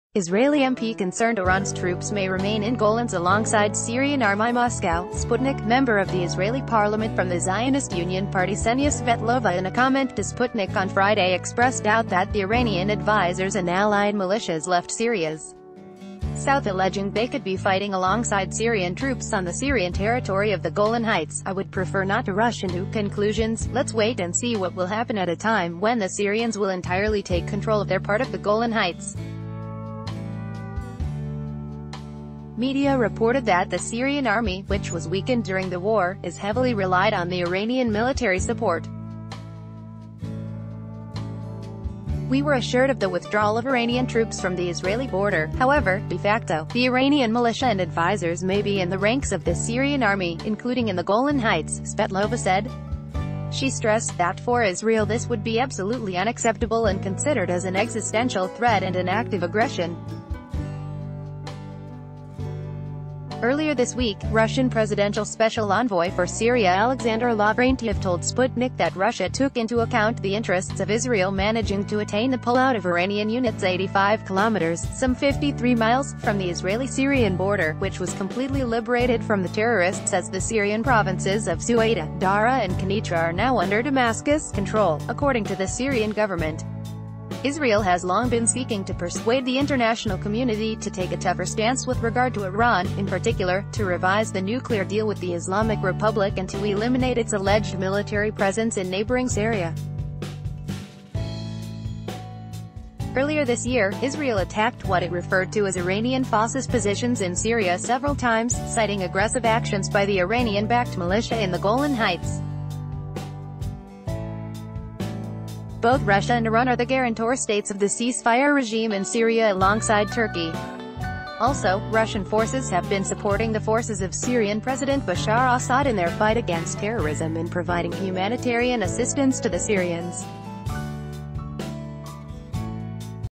Israeli MP Concerned Iran's Troops May Remain in Golans Alongside Syrian Army
Mathew Paul